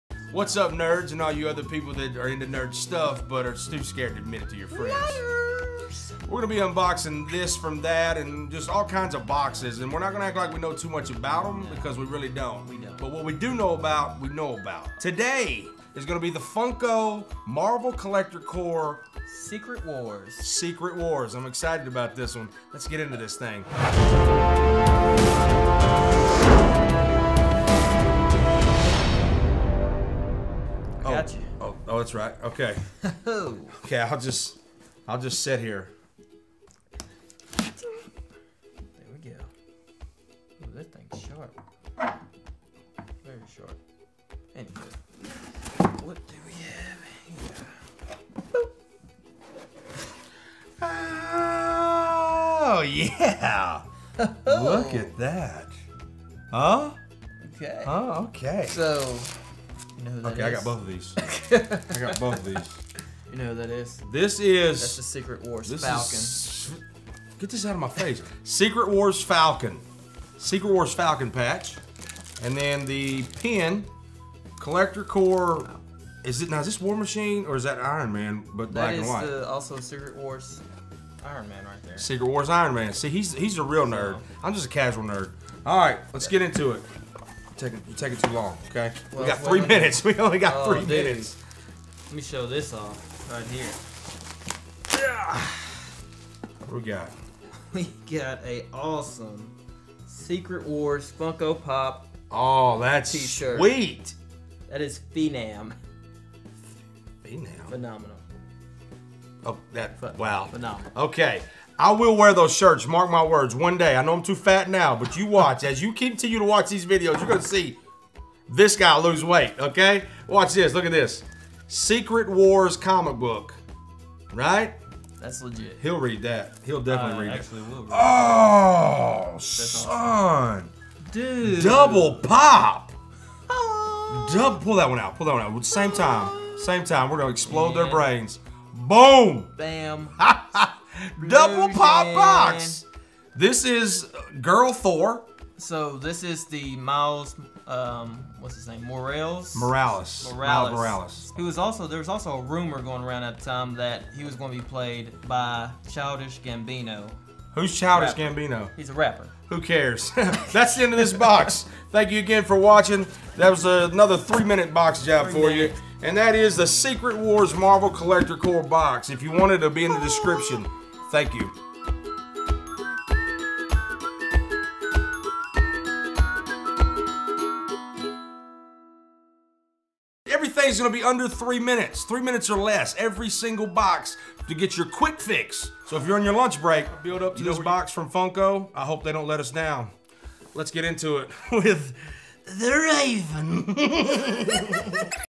DOUBLE POPS! Marvel Collector Corps SECRET WARS!
In this video we'll be unboxing the FUNKO Marvel Collector Corps SECRET WARS subscription box!! AWESOME BOX! As with every video we'll do it in 3 minutes or less, come on get your unbox fix! Love ya!